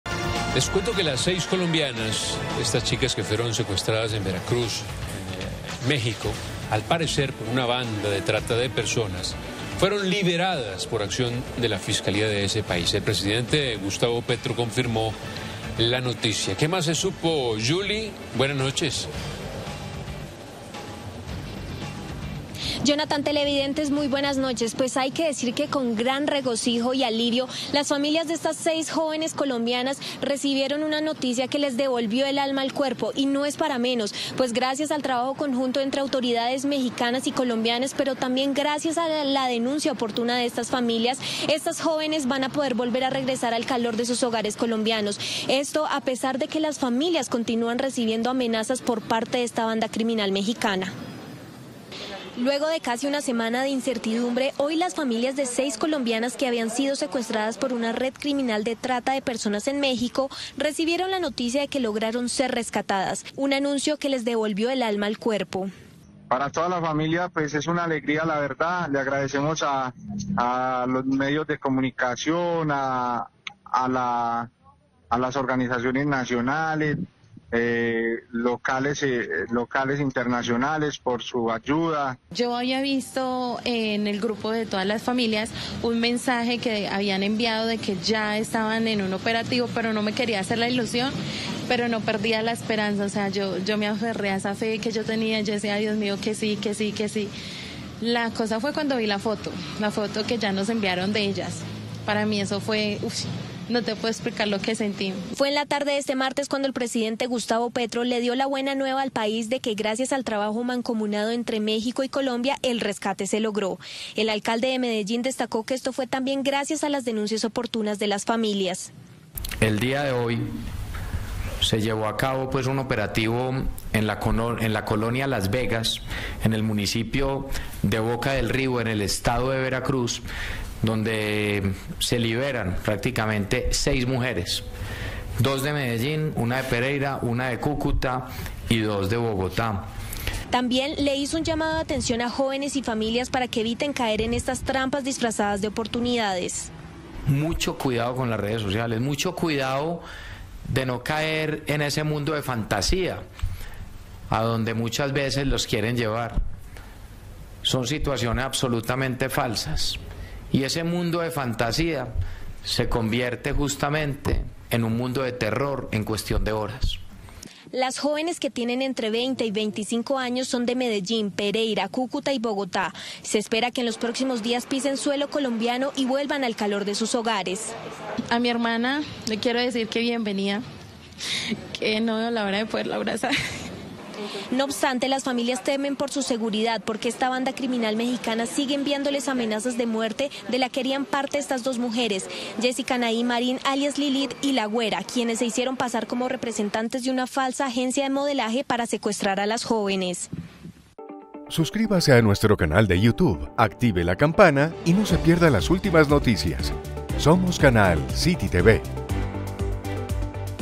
Fueron rescatadas las 6 mujeres colombianas secuestradas en México | CityTv
Buenas noticias para el territorio nacional. Las seis colombianas que fueron secuestradas en Veracruz, México, al parecer, por una banda de trata de personas, fueron liberadas por acción de la Fiscalía de ese país. Las víctimas habrían sido engañadas por dos mujeres, presuntamente miembros de una organización criminal, quienes se hicieron pasar como representantes de una falsa agencia de modelaje. Entérese de todos los detalles y conozca la información ampliada en la nota presentada a continuación.